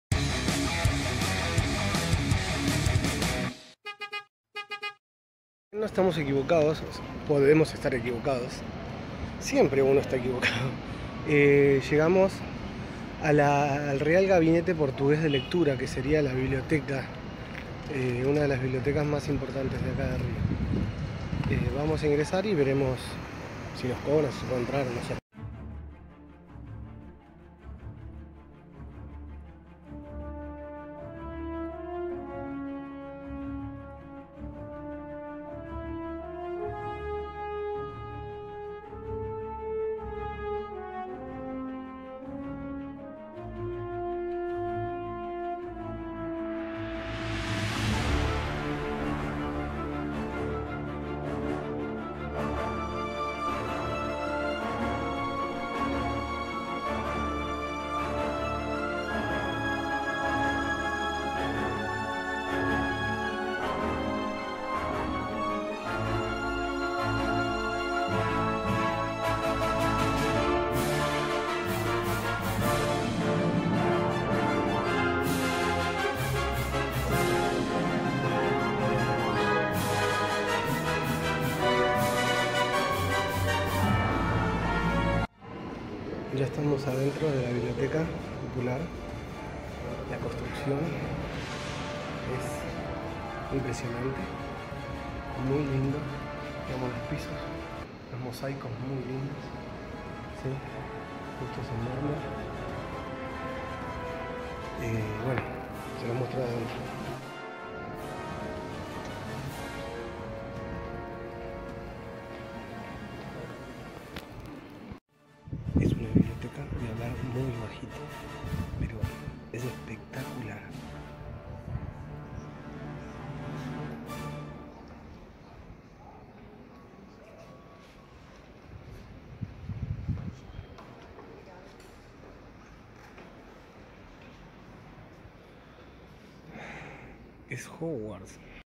La biblioteca de HARRY POTTER en Rio de Janeiro
El Real Gabinete Portugués de Lectura es una biblioteca y centro cultural de Río de Janeiro en Brasil. En su interior, destaca el Salón de Lectura, que se concibe en términos fastuosos y muy ornamentados en estilo neomanuelino.  Fue fundada en 1837 por un grupo de cuarenta y tres inmigrantes portugueses, refugiados políticos, para promover la cultura entre la comunidad portuguesa en la entonces capital del Imperio de Brasil.
El emperador Pedro II de Brasil (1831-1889) colocó la primera piedra del edificio el 10 de junio de 1880, y su hija Isabel, princesa imperial de Brasil, junto con su marido, el príncipe Gastón, conde de Eu, lo inauguraron el 10 de septiembre de 1887.